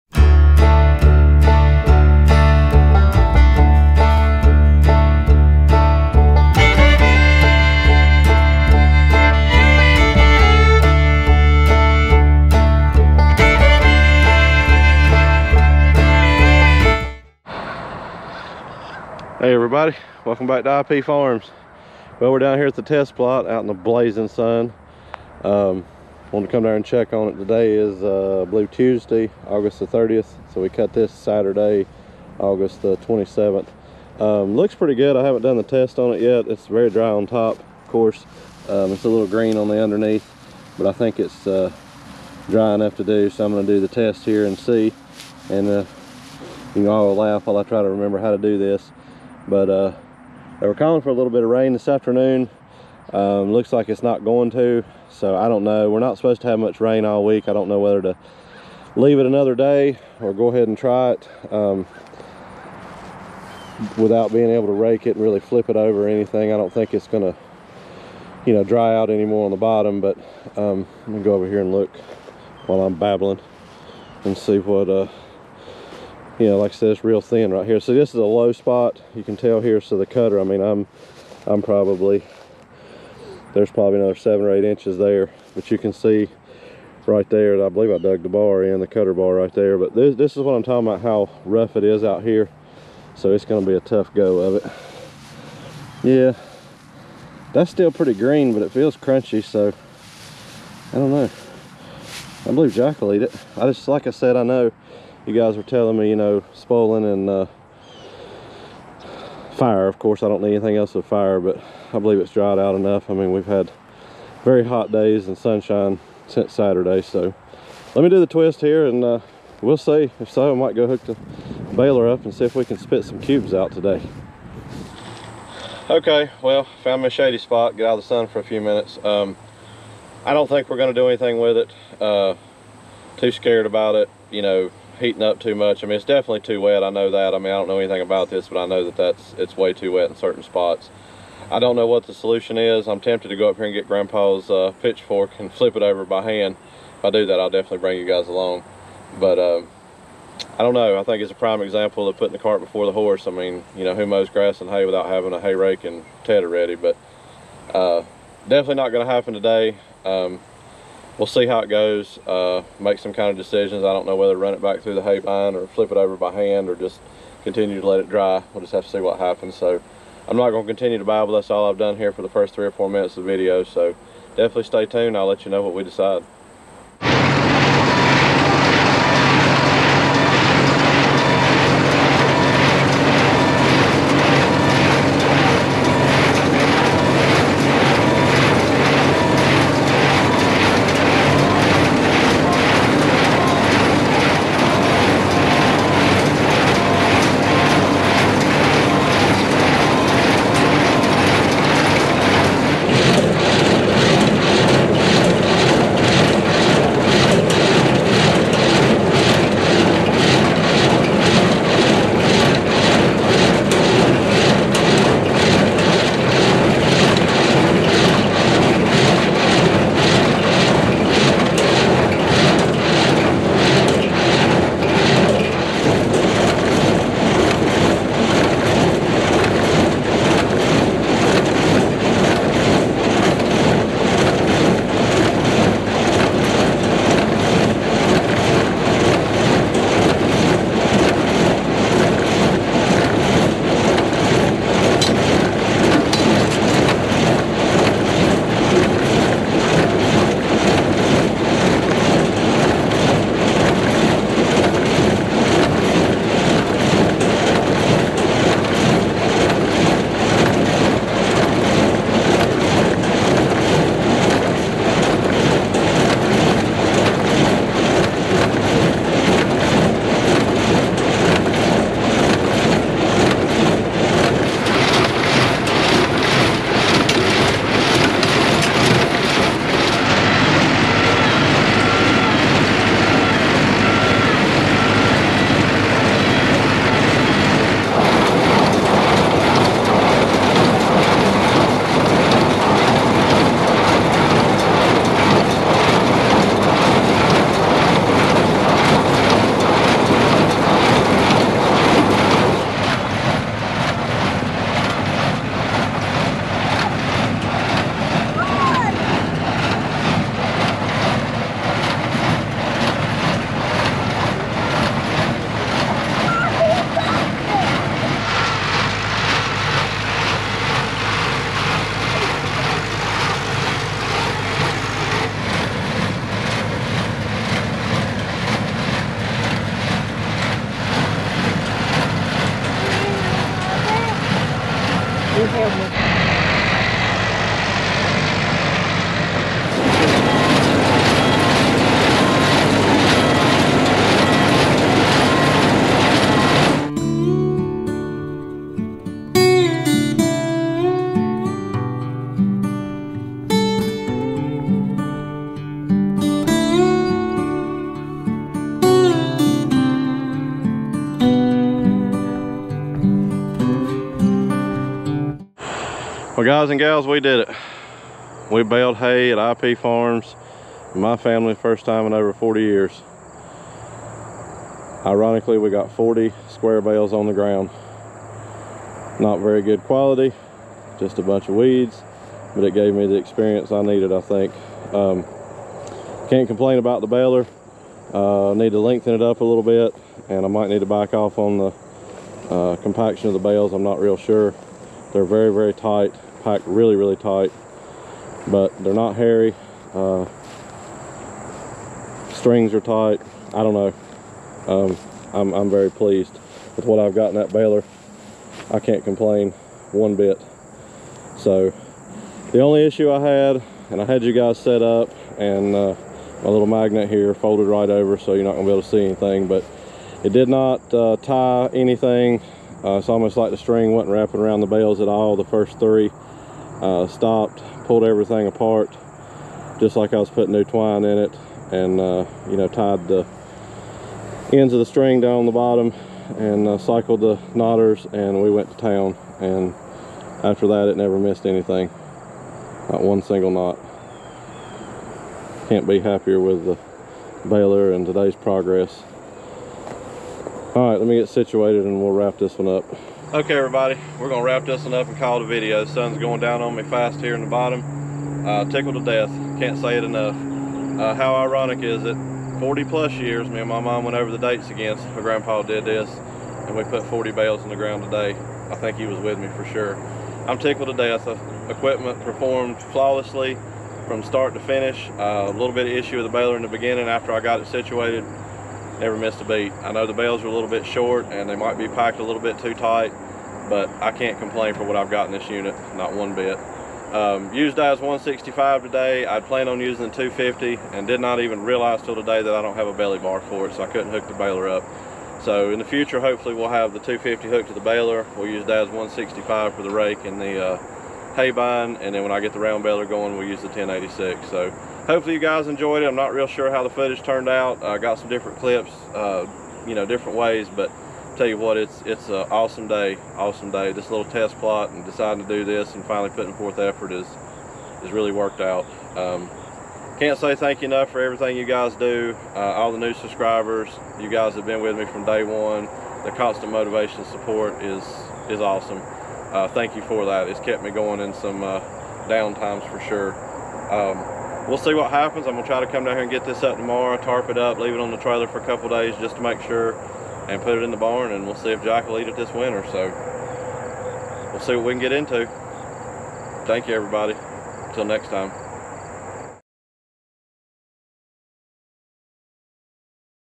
Let's bale some HAY
Making hay like it was 1975 again, all went well and think we are finally farming!